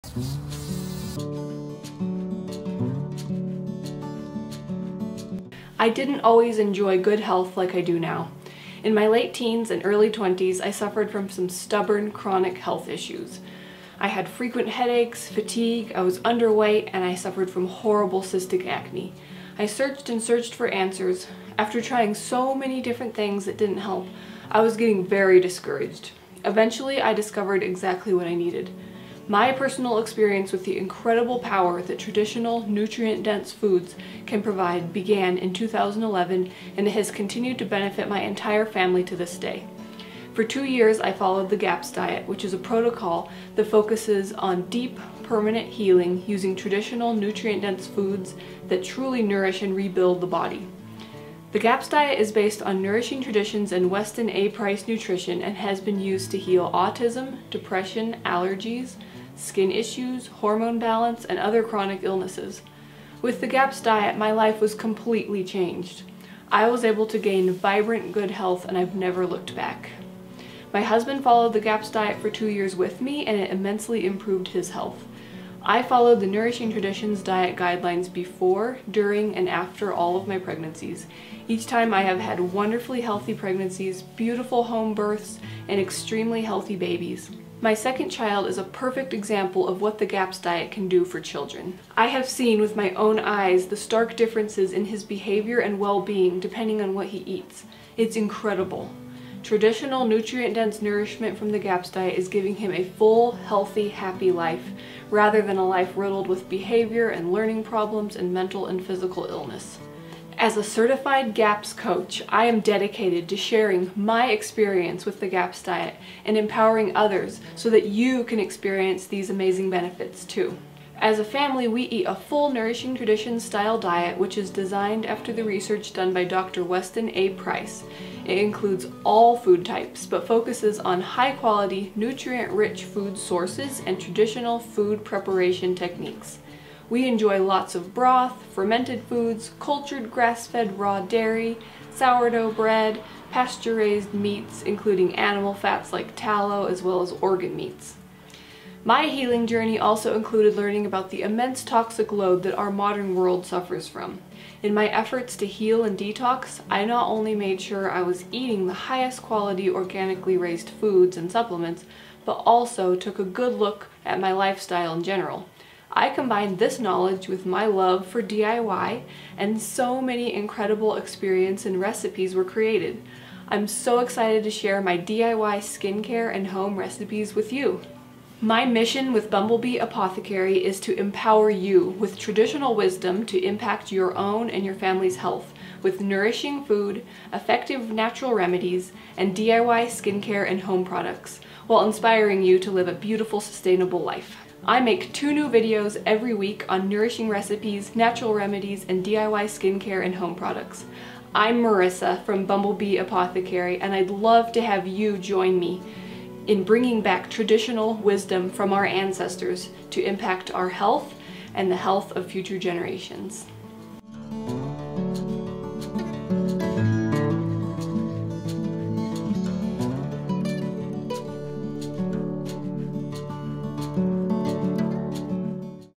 Traditional Wisdom in Nourishment and Natural Living with Bumblebee Apothecary | MOM VLOG HEALTHY
⬇⬇⬇Click “SHOW MORE“ for more info⬇⬇⬇
Come along as I share my Bumblebee Apothecary story.

🐝 SHOP THIS VIDEO:
“GAPS, Stage by Stage, with Recipes” by Becky Plotner

-Marisa 🐝

GAPS™ and Gut and Psychology Syndrome™ are the trademark and copyright of Dr. Natasha Campbell-McBride.